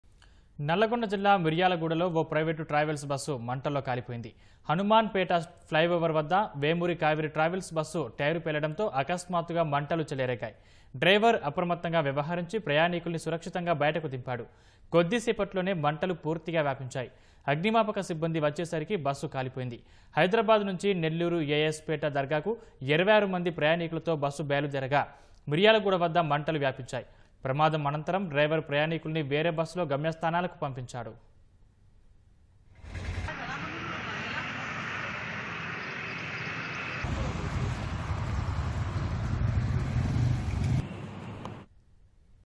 Private Travel Bus Catches Fire at Miryalaguda | మిర్యాలగూడలో మంటల్లో కాలిన ప్రైవేటు ట్రావెల్స్ బస్
నల్లగొండ జిల్లా మిర్యాలగూడలో ఓ ప్రైవేటు ట్రావెల్స్ బస్సు మంటల్లో కాలిపోయింది. హనుమాన్ పేట ఫ్లైఓవర్ వద్ద వేమూరి కావేరి ట్రావెల్స్ బస్సు టైరు పేలడంతో అకస్మాతుగా మంటలు చెలరేగాయి. డ్రైవర్ అప్రమత్తంగా వ్యవహరించి ప్రయాణికులను సురక్షితంగా బయటకు దింపాడు. కొద్దిసేపట్లోనే మంటలు పూర్తిగా వ్యాపించాయి. అగ్నిమాపక సిబ్బంది వచ్చేసరికి బస్సు కాలిపోయింది. హైదరాబాద్ నుంచి నెల్లూరు A.S.పేట దర్గాకు 26 మంది ప్రయాణికులతో బస్సు బయలుదేరగా. మిర్యాలగూడ వద్ద మంటలు వ్యాపించాయి. ప్రమాదం అనంతరం డ్రైవర్ ప్రయాణికులను వేరే బస్సులో గమ్యస్థానాలకు పంపించాడు.